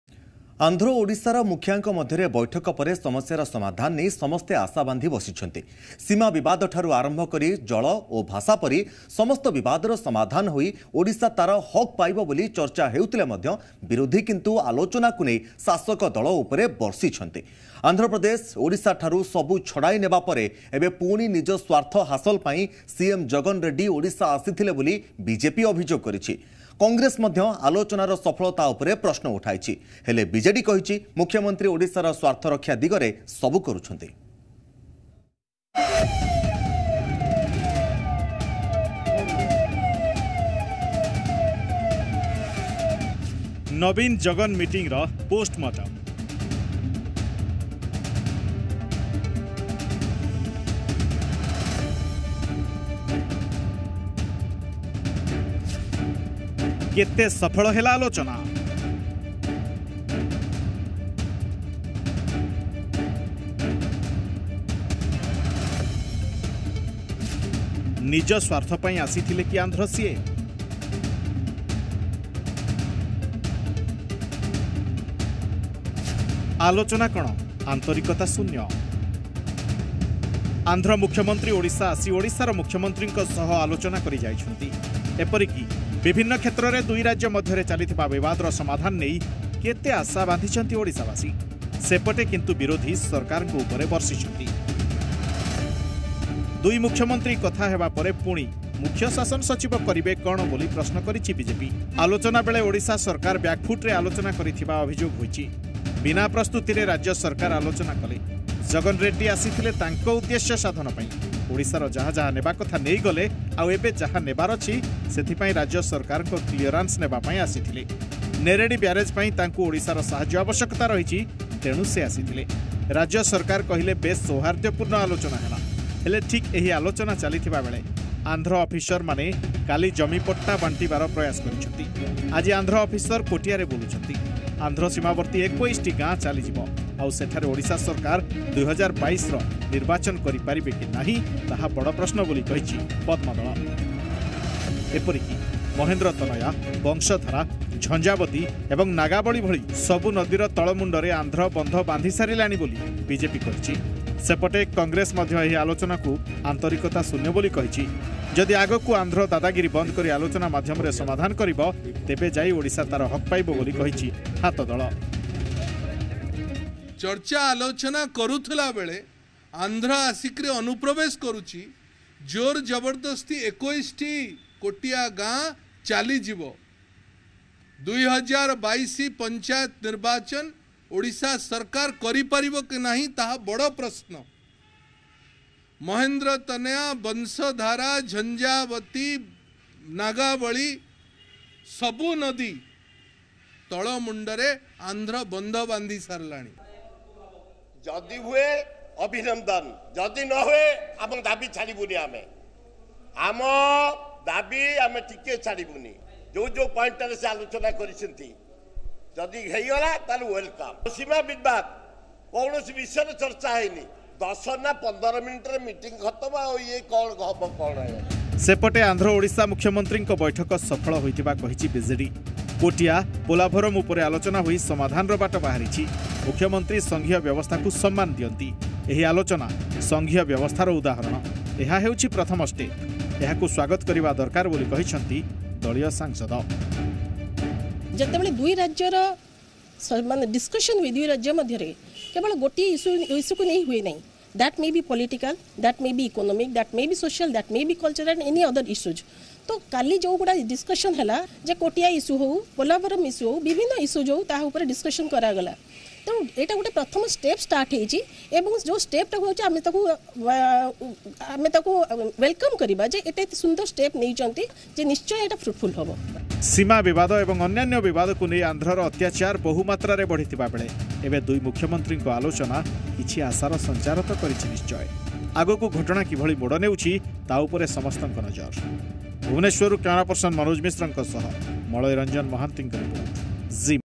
Odisha Bjp Targets Cm Naveen Patnaik | ନବୀନ-ଜଗନ ମିଟିଂର ପୋଷ୍ଟମର୍ଟମ | Zee Odisha | Big News
About Channel:-